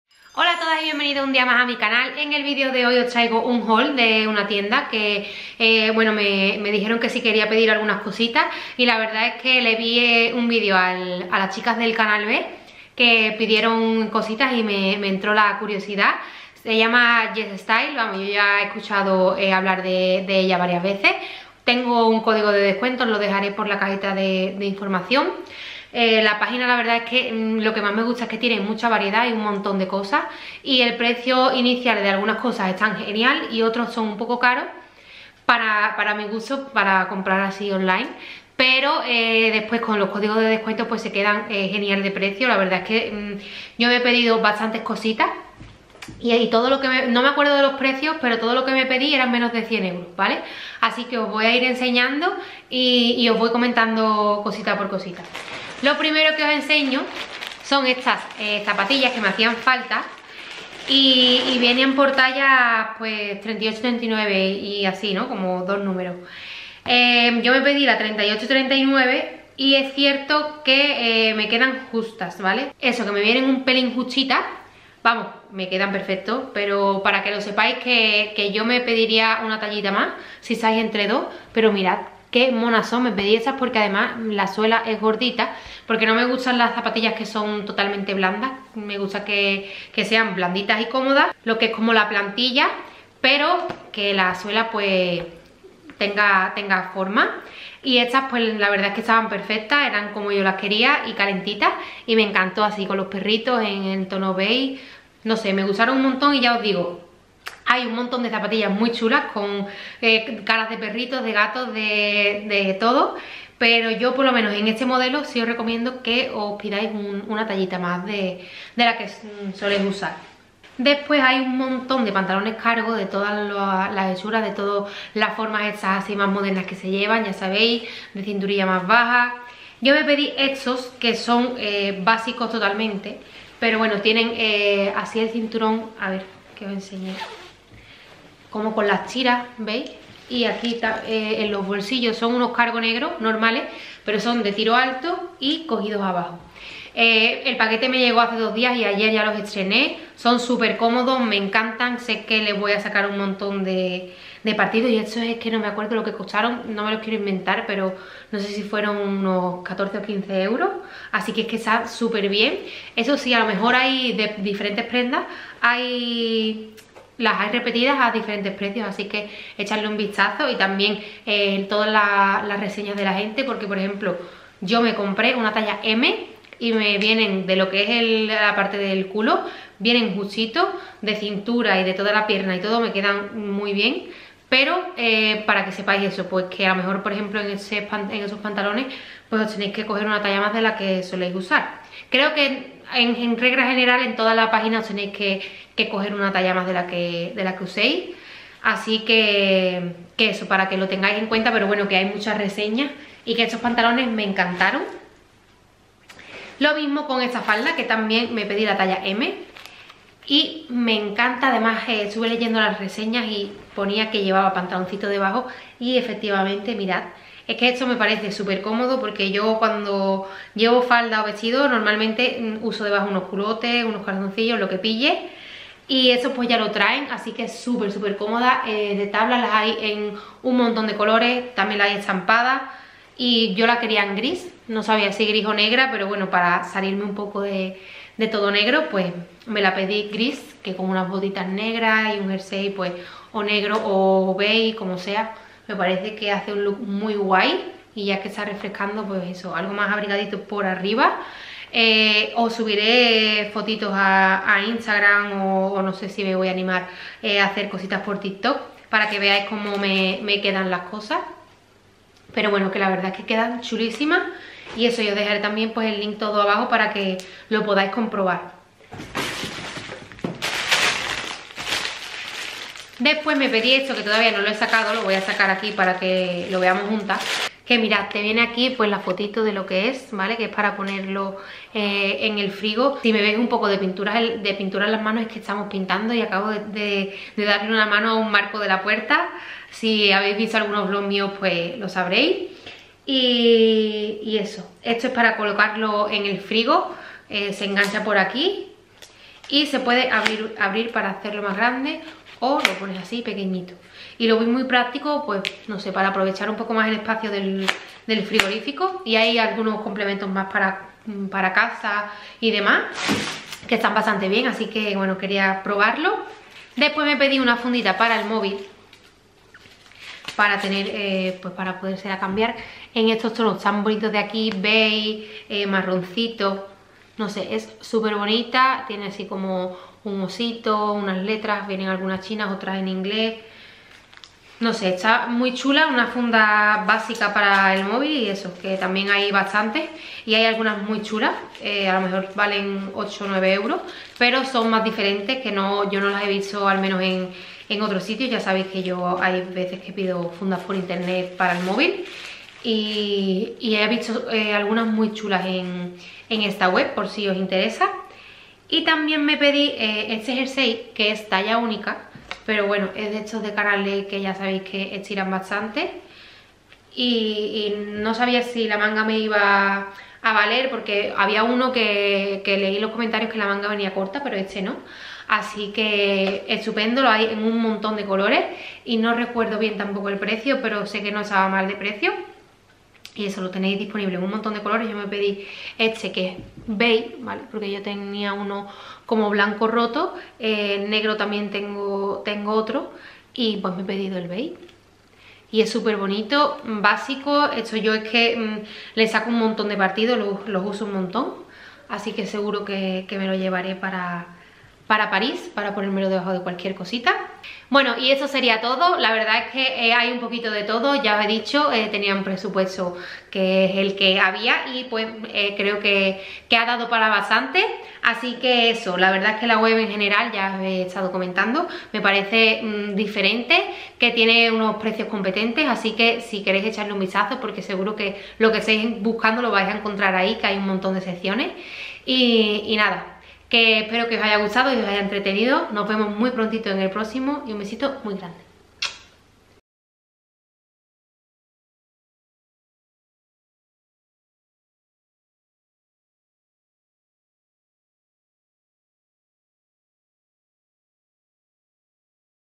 haul nueva temporada ropa y accesorios
hola!!! en el vídeo de hoy os traigo un haul de una tienda diferente de la que había oído hablar pero nunca había pedido nada
vi un pedido que hicieron las chicas del canal be y me contactaron para mandarme cositas así que aquí os traigo lo que me pedí

Código CEAEME15